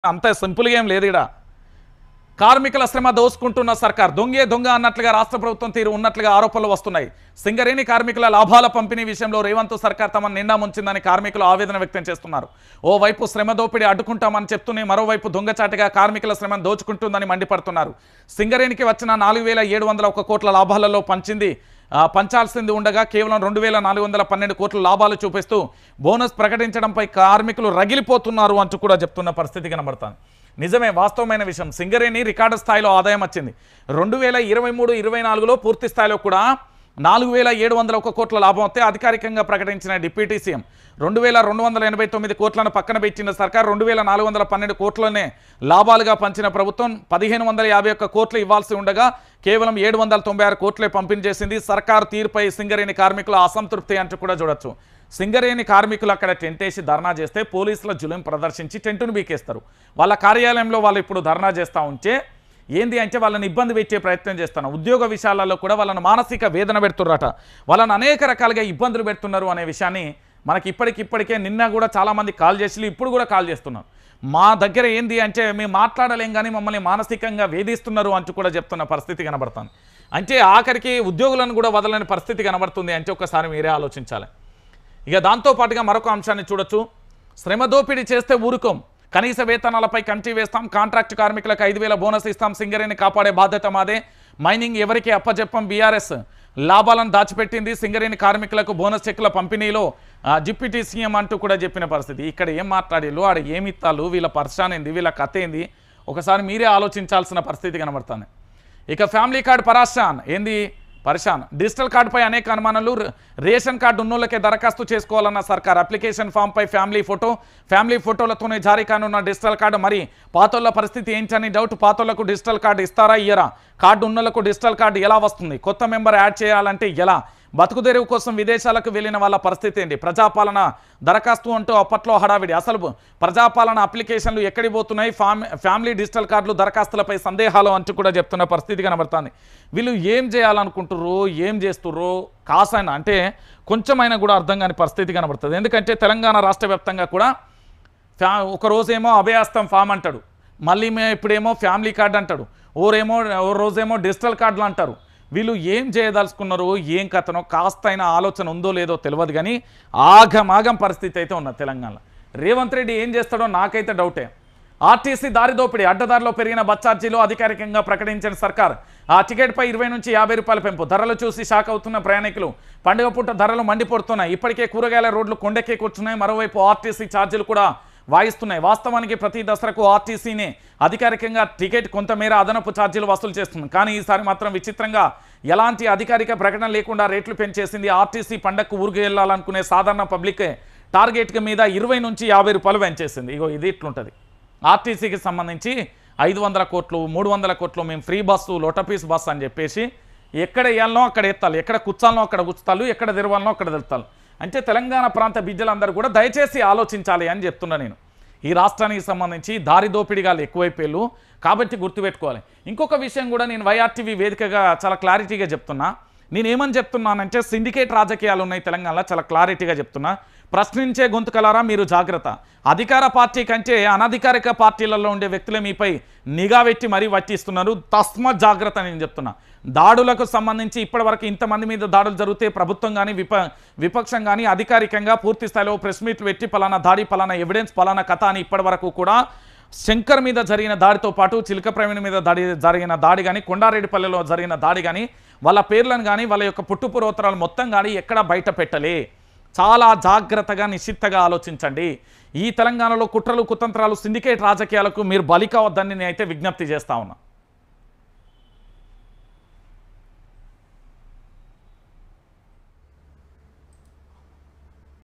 Singareni Workers Fire on Congress Govt & Revanth Reddy Over Fake Promise on Bonus | YR TV Telugu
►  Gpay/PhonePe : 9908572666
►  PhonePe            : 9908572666